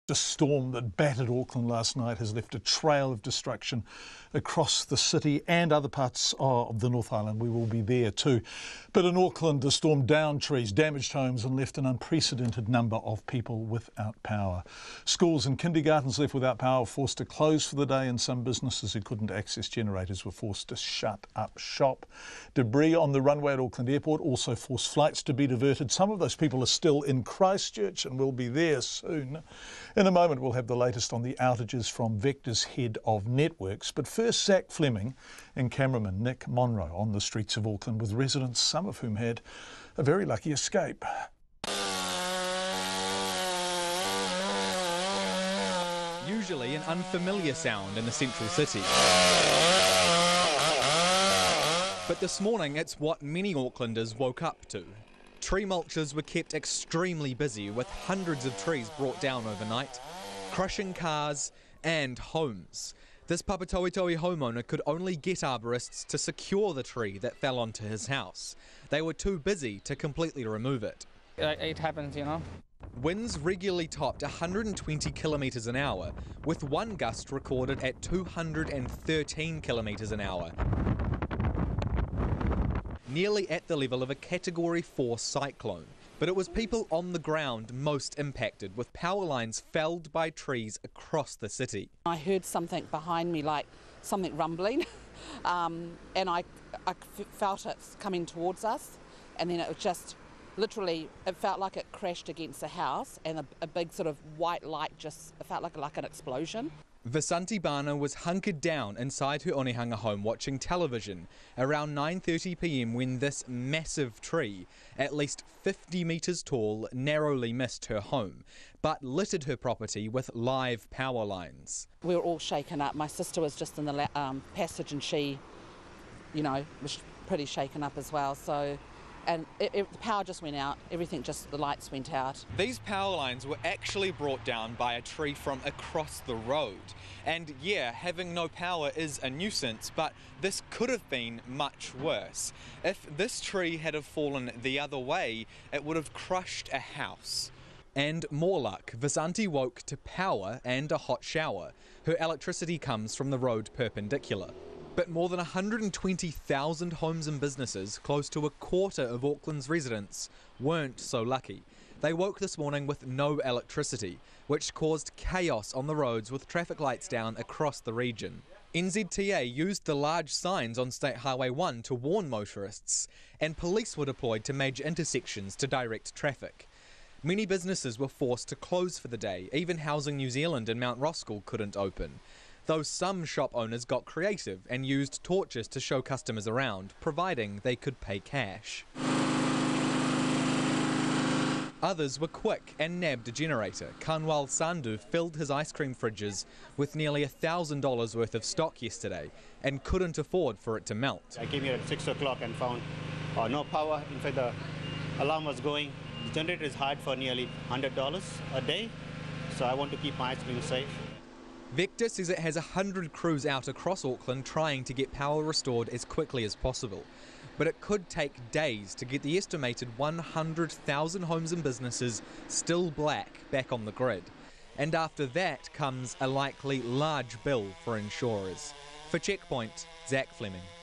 Chaos in Auckland after storm strikes overnight
Power was cut to 120,000 homes and businesses, traffic lights were out across the region, and trees crashed on to homes, cars and roads throughout the city. Zac Fleming reports.